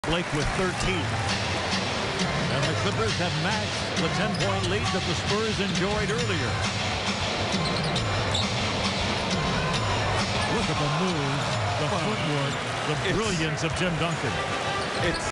Tim Duncan Takes DeAndre Jordan on a Spin Cycle
Vintage Timmy. December 16, 2013.